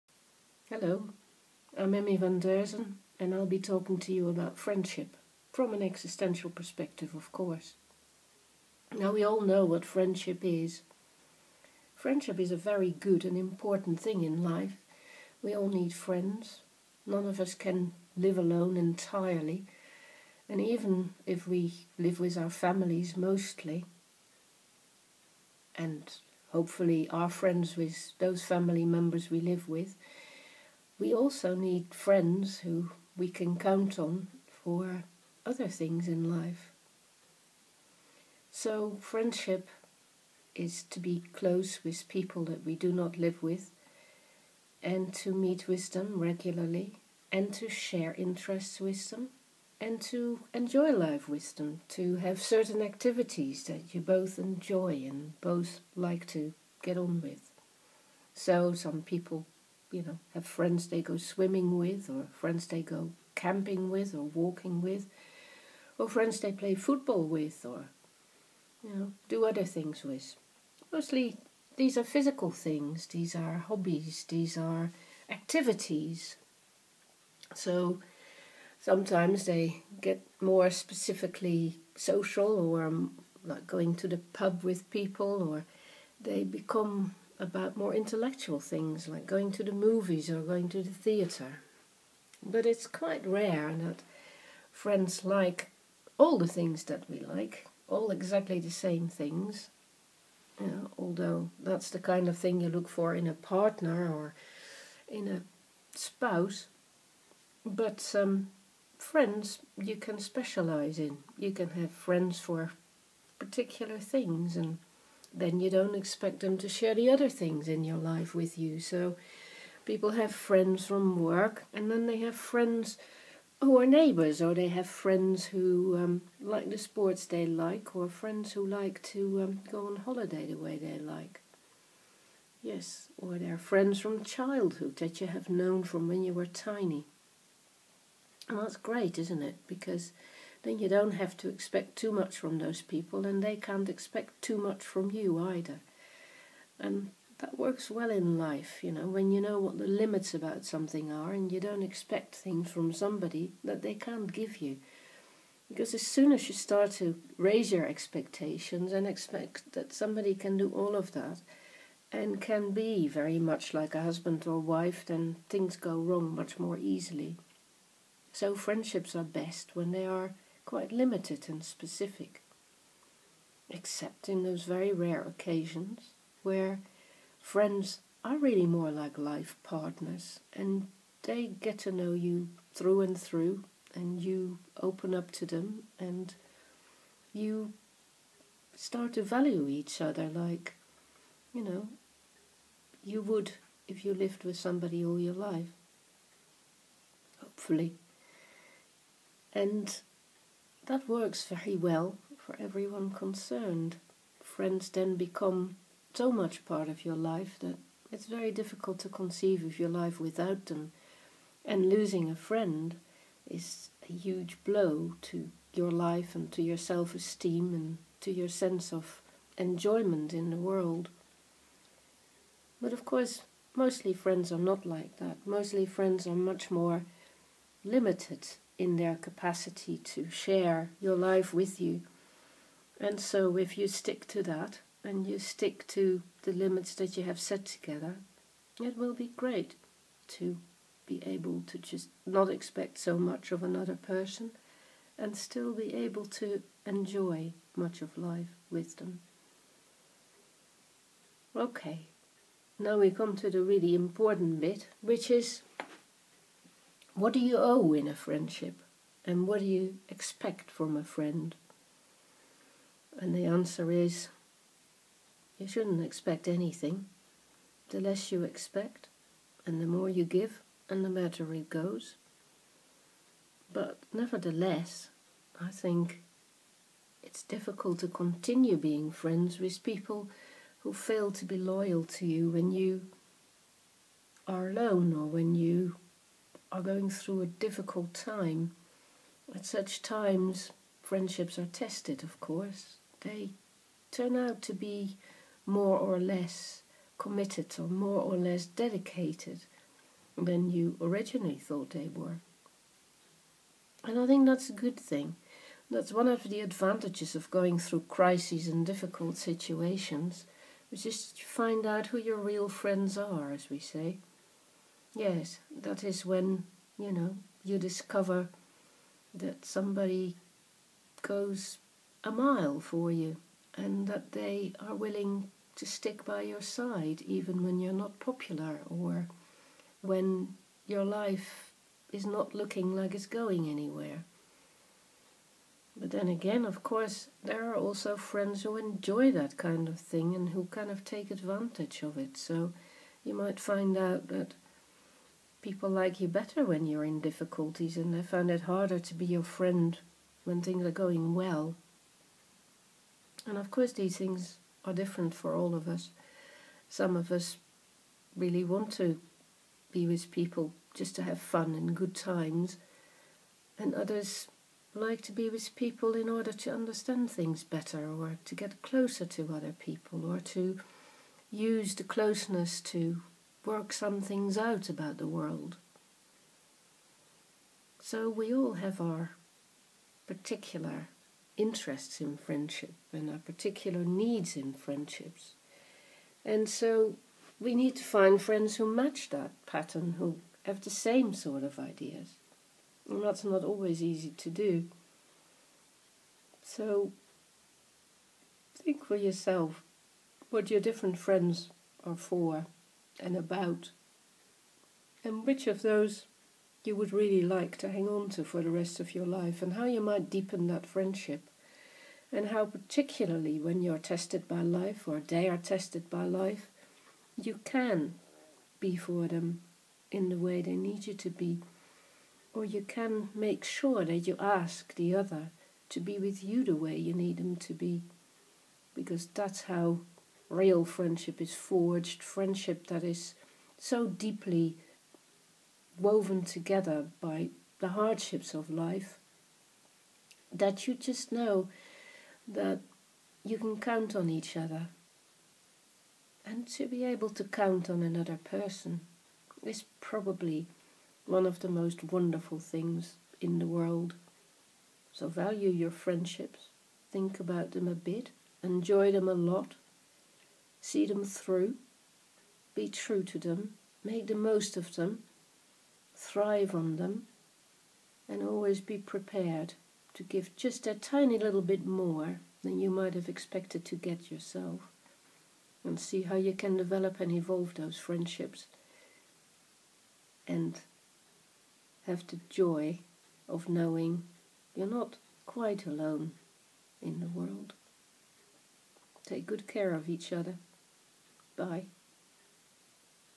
A few words about friendship
Existential therapist Emmy van Deurzen speaks of the importance of friendship in life.